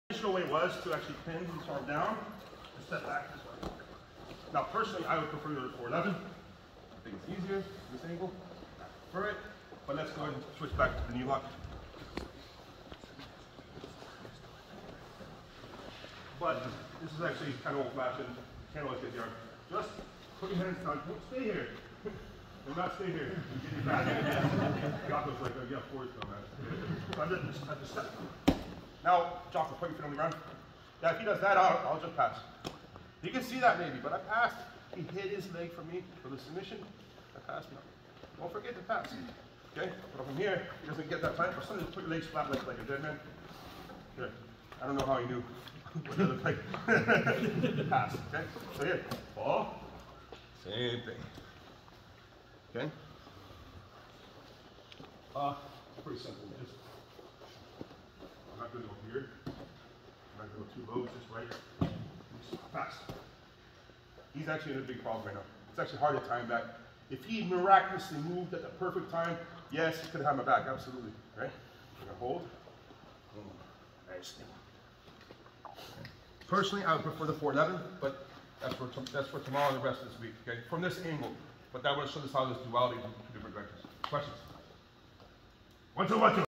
Dean Lister: NoGi Back Step Knee Lock from Opponents Half Guard
Dean Lister shares a knee lock and pass from inside an opponents half guard at Origin Immersion Camp 2022.